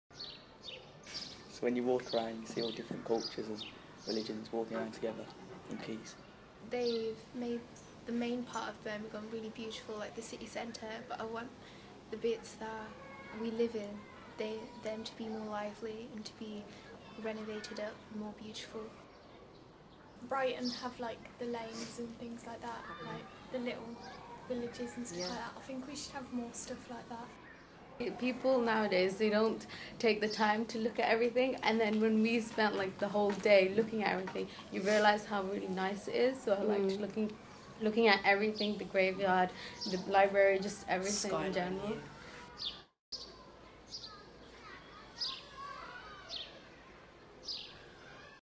Trailblazers: Ninestiles
An animation by Anita Shukla using the photographs and voices of students from Ninestiles school sharing their perspective on the City of Birmingham.